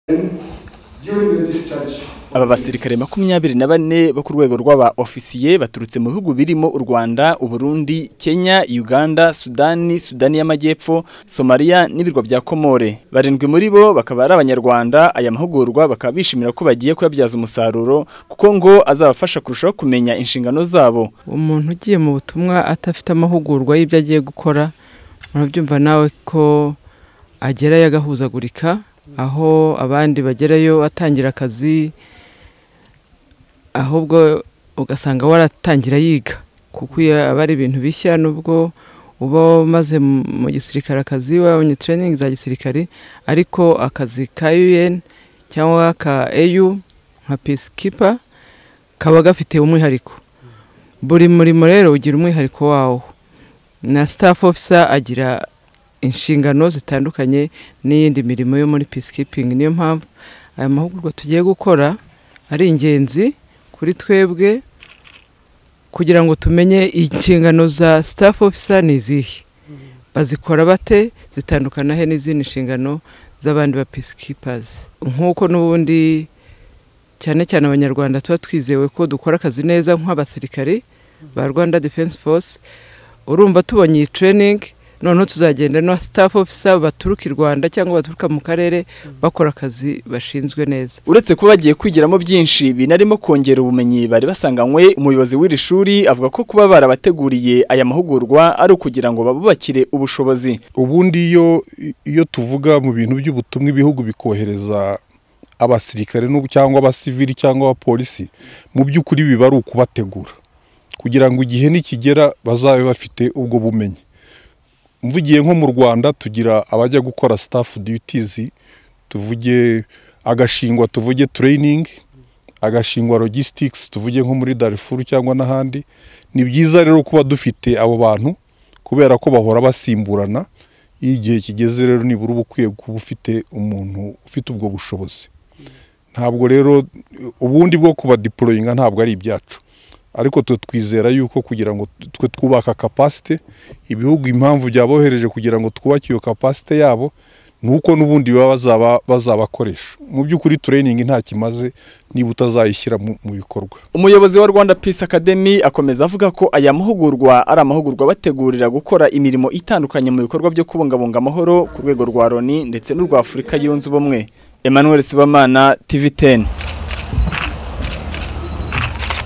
Amahugurwa y'aba Officiers muri Rwanda Peace Academy Kinya/MUSANZE
Kuri uyu wa mbere,mu ishuri rya Gisirikare rya Rwanda Peace Academy riherereye mu Karere ka MUSANZE hatangiye amahugurwa y'ibyumweru bibiri akaba ari guhabwa aba Officiers  24 baturutse mu bihugu 8 birimo n'u Rwanda,bikaba ari mu rwego rwo kubategura gukora imirimo irimo kubungabunga umutekano ku rwego rwa LONI n'urwa Africa yunze ubumwe.